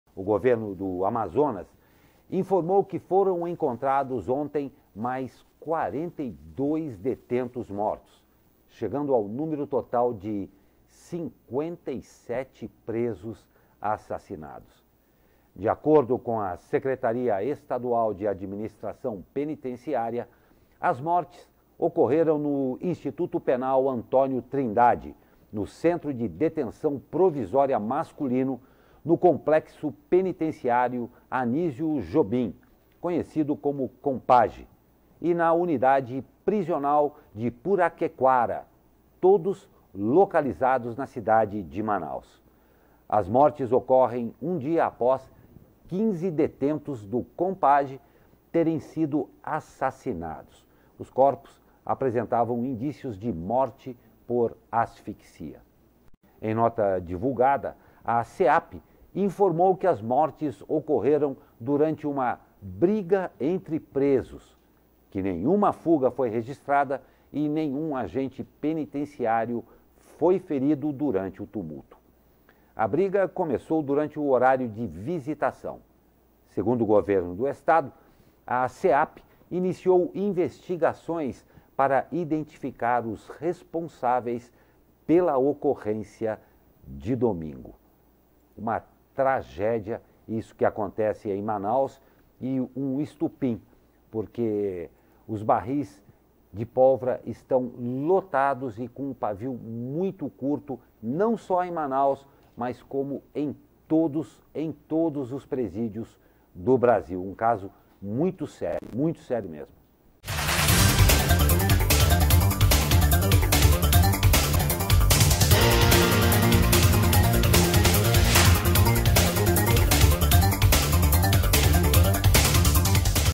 Governo do AM confirma 42 novas mortes em presídios; total chega a 57
De acordo com a secretaria estadual de Administração Penitenciária, as mortes ocorreram no Instituto Penal Antônio Trindade, no Centro de Detenção Provisória Masculino, no Complexo Penitenciário Anísio Jobim conhecido como Compaj e na Unidade Prisional do Puraquequara, todos localizados na cidade de Manaus.